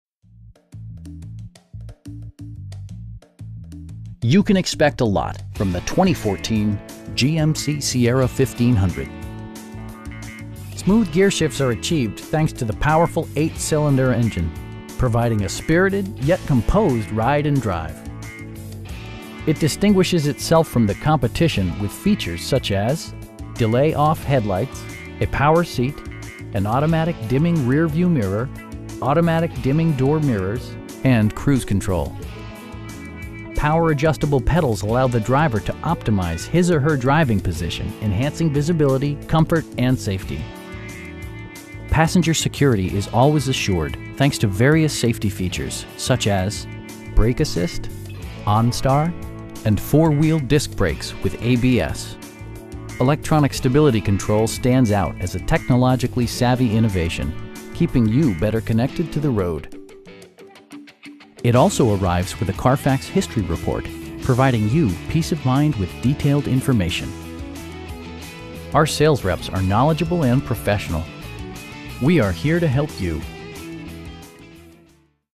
2014 GMC Sierra 1500 SLT in Conroe, TX 77304
You can expect a lot from the 2014 GMC Sierra 1500. Smooth gearshifts are achieved thanks to the powerful 8 cylinder engine, providing a spirited, yet composed ride and drive. It distinguishes itself from the competition with features such as: delay-off headlights, a power seat, an automatic dimming rear-view mirror, automatic dimming door mirrors, heated door mirrors, an overhead console, and cruise control. Features such as automatic climate control and leather upholstery prove that economical transportation does not need to be sparsely equipped. Power adjustable pedals allow the driver to optimize his or her driving position, enhancing visibility, comfort and safety. Passenger security is always assured thanks to various safety features, such as: dual front impact airbags with occupant sensing airbag, front side impact airbags, traction control, brake assist, a security system, onStar, and 4 wheel disc brakes with ABS. Electronic stability control stands out as a technologically savvy innovation, keeping you better connected to the road. It also arrives with a Carfax history report, providing you peace of mind with detailed information. Our sales reps are knowledgeable and professional. We'd be happy to answer any questions that you may have. We are here to help you.